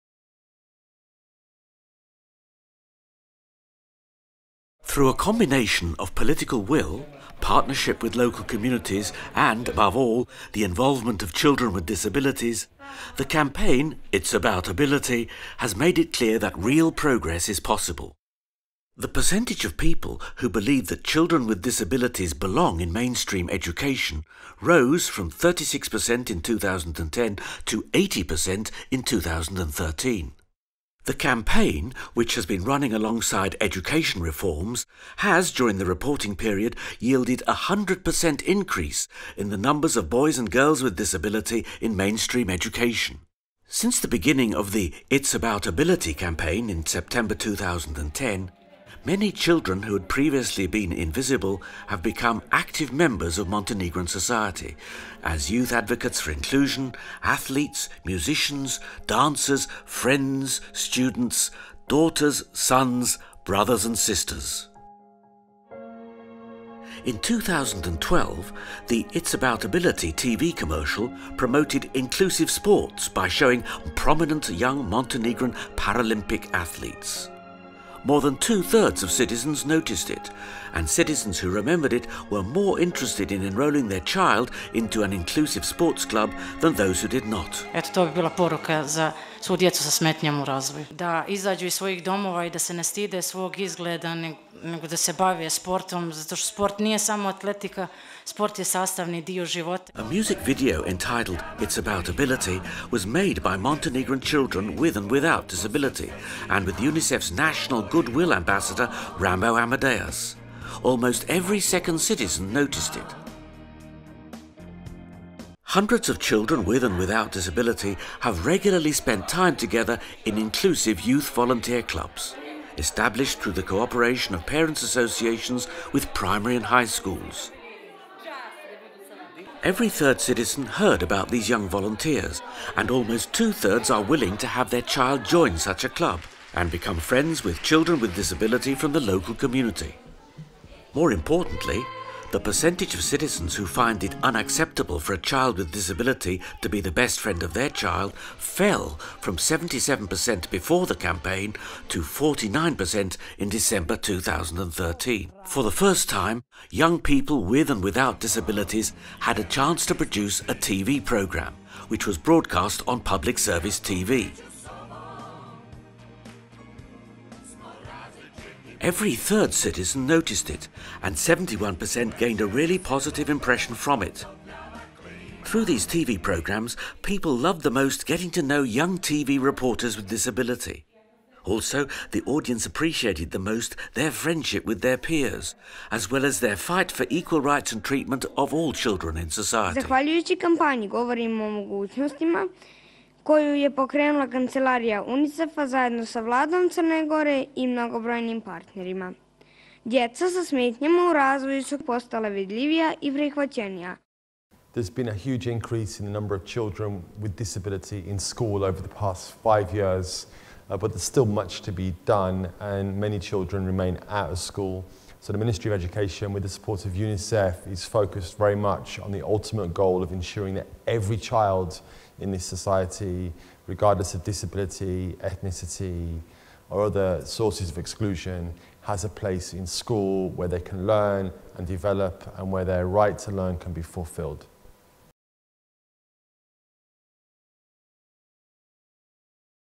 Innovative Policy 2016: Advocacy programme “It’s about Ability” | Montenegro
UNICEF Montenegro’s “It’s about Ability” programme – a nationwide campaign from 2010 to 2014 – significantly improved the attitudes of Montenegrins towards Inclusive Education. Whereas in 2010 only 35% of the population found it acceptable for a child with a disability to attend the same class as their own non-disabled child, this figure increased to 78% in 2015.